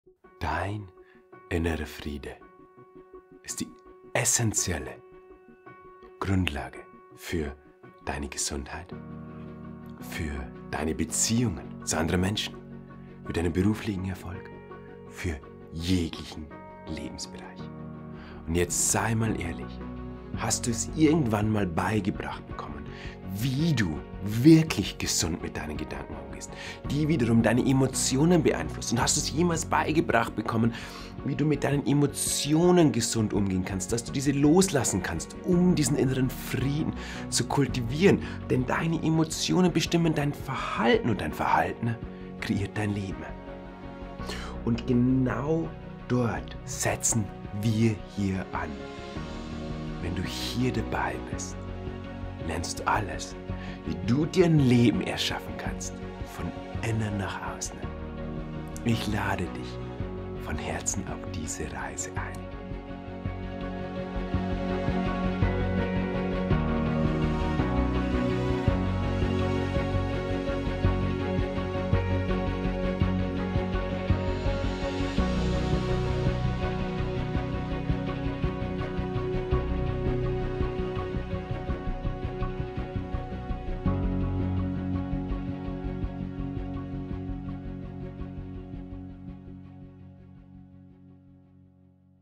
Willkommen in der Achtsamkeits-Akademie!
Dein innerer Frieden ist die Voraussetzung für deine Gesundheit, deine Beziehungen, deinen beruflichen Erfolg, dein ganzes LEBEN! Die Academy besteht aus weit mehr als nur Online-Kursen. Sie ist eine Gemeinschaft von tausenden Menschen, die gemeinsam auf dem gleichen Weg sind wie du!

Es warten auf dich für nur 1 Euro im ersten Monat:

▶︎ Ein tiefgreifender Online-Kurs, der dir dabei hilft, Achtsamkeit in deinem Leben zu verankern.
▶︎ Zugang zu einer riesigen Meditations-Bibliothek
▶︎ 3 Live Webinare mit mir, wenn du dich noch vor dem 21. März anmeldest! (insgesamt 8 in den nächsten 11 Wochen)
▶︎ Zugang zur exklusiven Facebook Gruppe
▶︎ Optional bestellbar: Dein liebevoll gestaltetes Workbook mit über 190 Seiten

Melde dich jetzt noch bis zum 21. März um 23:59 an und sichere dir deinen Probemonat mit deinen optionalen Kursunterlagen! Danach werden die Tore geschlossen und wir starten mit frischer Energie und den neuen wundervollen Mitgliedern in ein neues Leben!

Bist du dabei in der Academy? Welche Veränderung wünschst du dir so sehr in deinem Leben?

Herzlich, dein Peter 😊